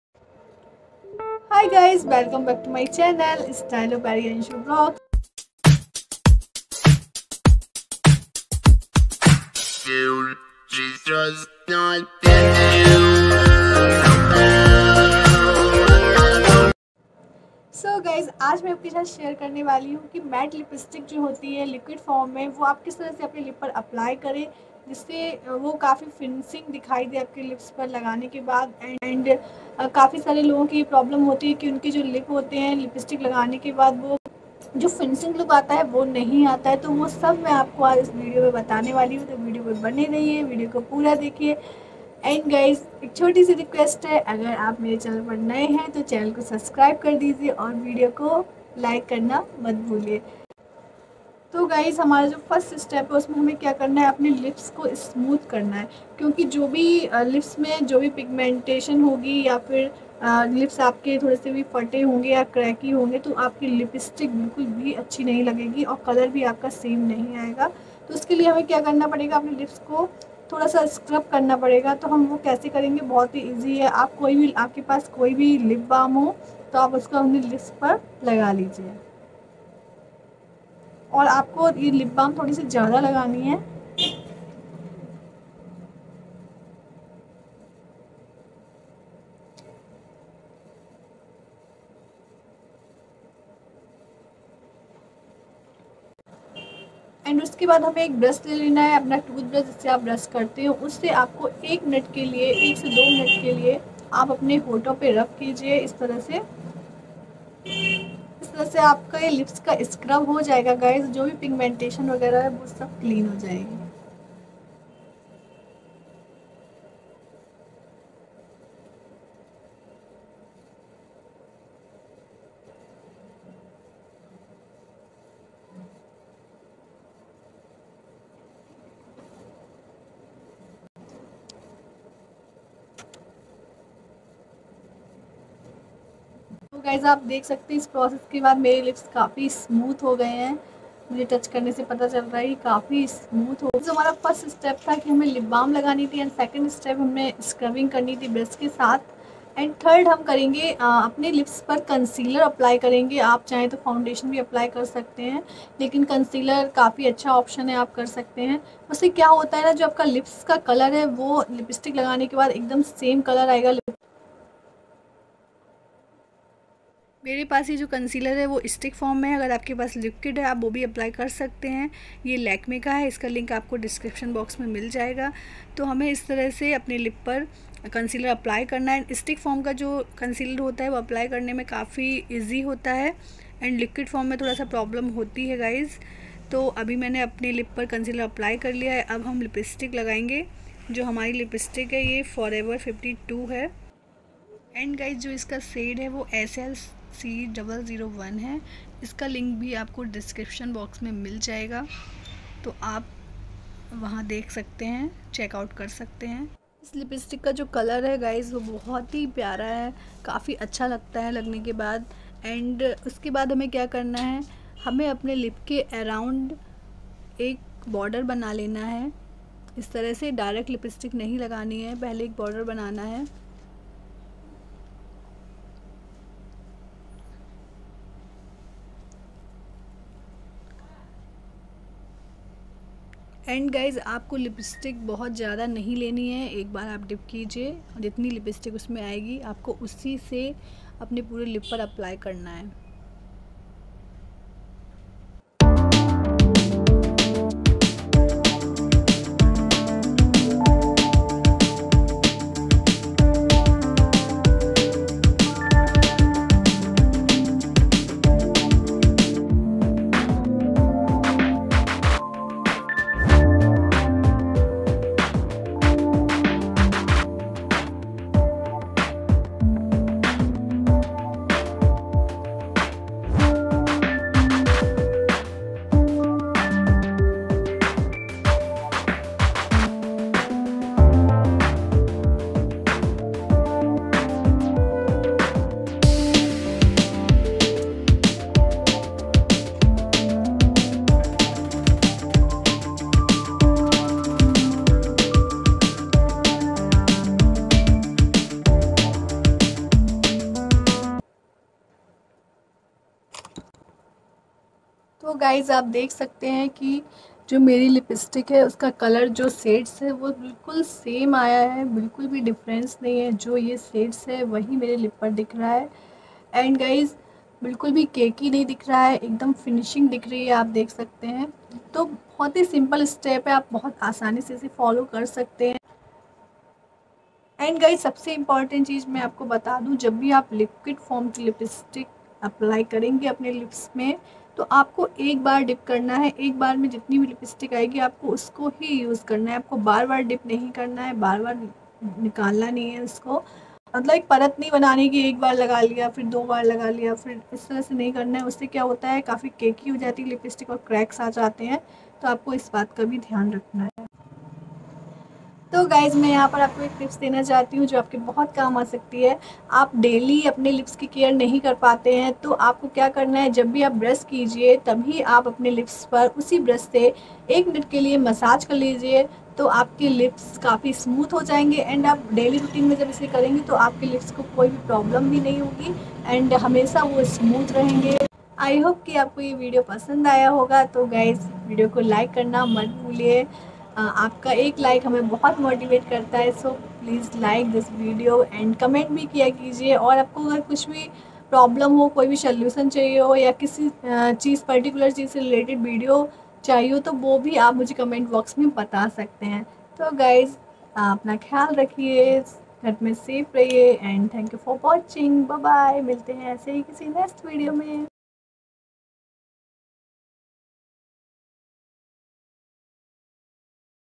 How to apply liquid lipstick perfectly ! लिपस्टिक लगाने का सही तरीका! टिप्स !Stylopairi Anshu Vlogs
आज मैं आपके साथ शेयर  kar rhi hun aap met liquid lipstick ko kis trh se lgaye jo bo bilkul perfectly lge or kin bato ka dhyan rkhe lipstic lgate smy men aaj aapko es video ke madhyam se kuch tips bhi dena chahunga jinko dhyan mai rakhkar aap agar lipstick apply kregi to aapko lipstick kabhi bhi khrab trike se nhi lgegi or kafi long lastic chalegi !!